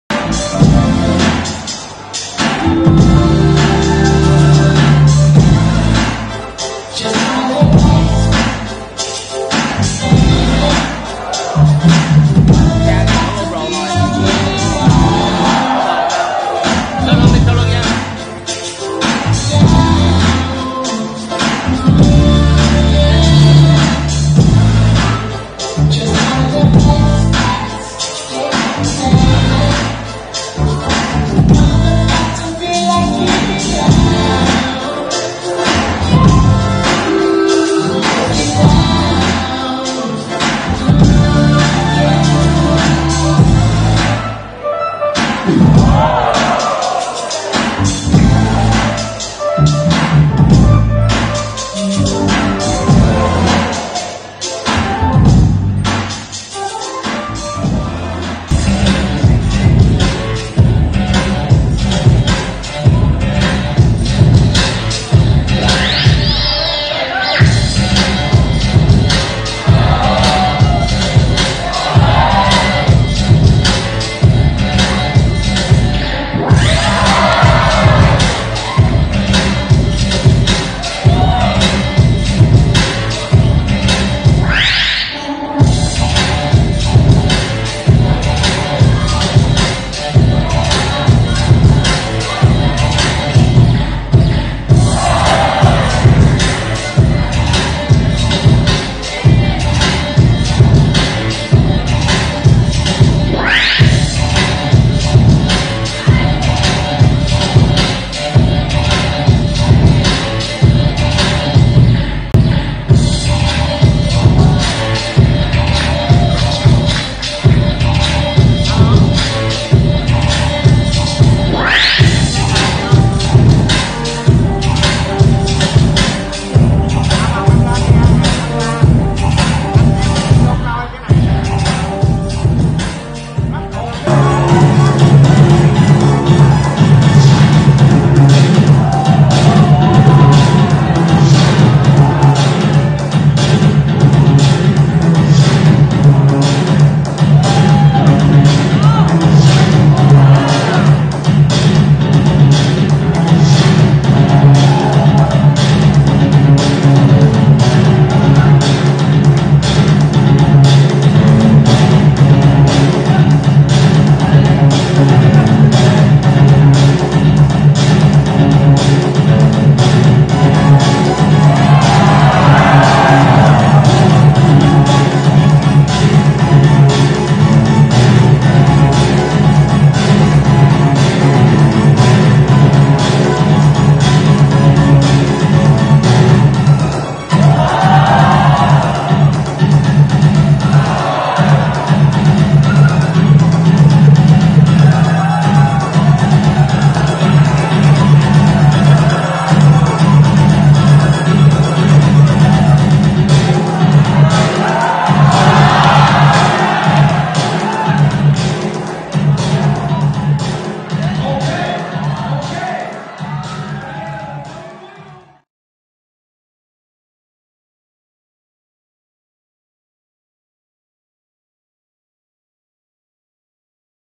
HOZIN's AMAZING ROUNDS Against GREENTECK (2016)
HOZIN battled Greenteck at Together Time 2016

✮ This Channel documents and showcases the GREATEST POPPERS of all time

✮ Click into channel to view more WORLD-CLASS PERFORMANCES